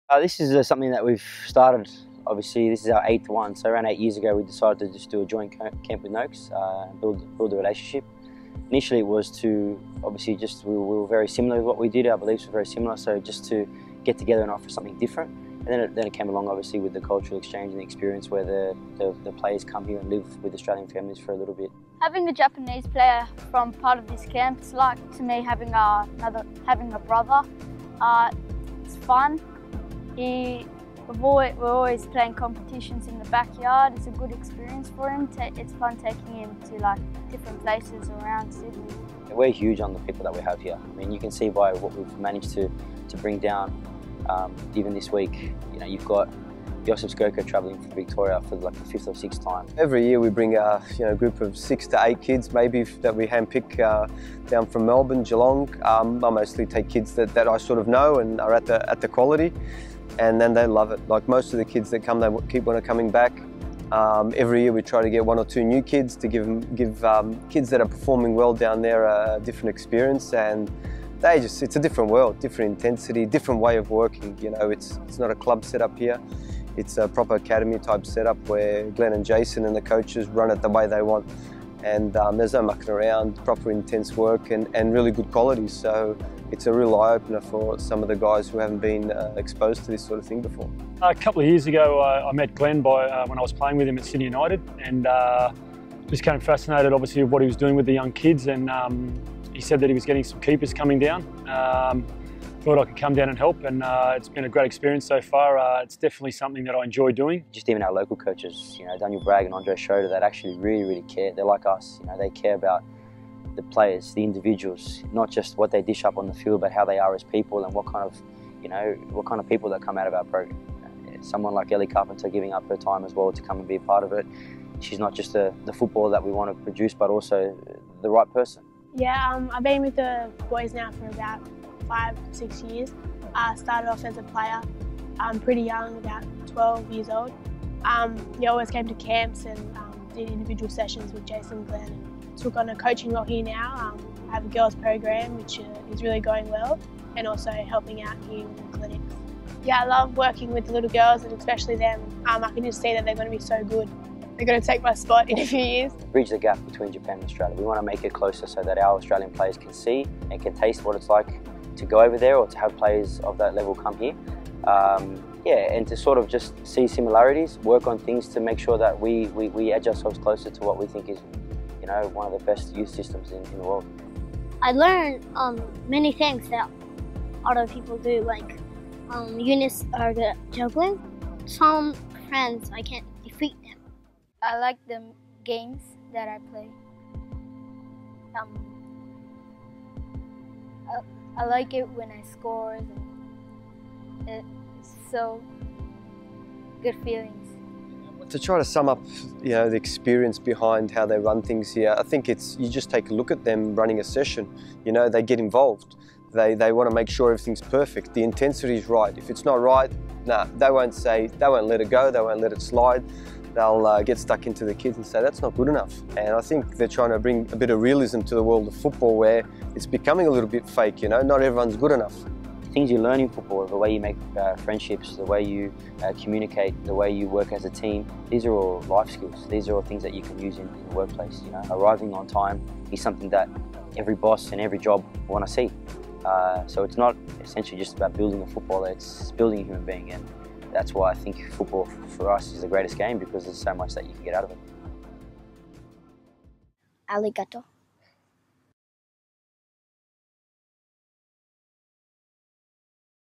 Futboltec and TOC Partnership 2017
Insight in to our Futboltec and TOC partnership. The way we work, our focus and the progression over the last 8 years. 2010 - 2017